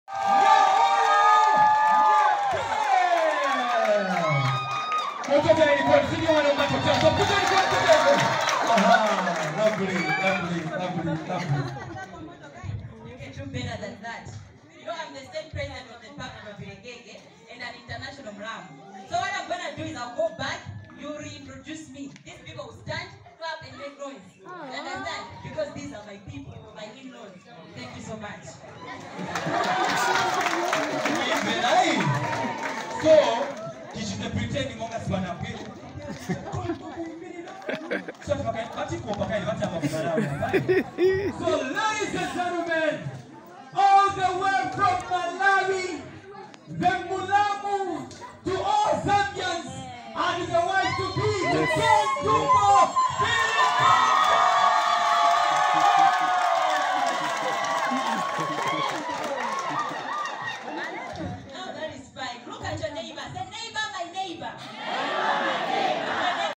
Nyauyu in Zambia
PLEASE NOTE: Unauthorized upload of this video unto any YouTube channel is prohibited. (C) 2022. VILEKEKE. People excited to see Nyauyu in Zambia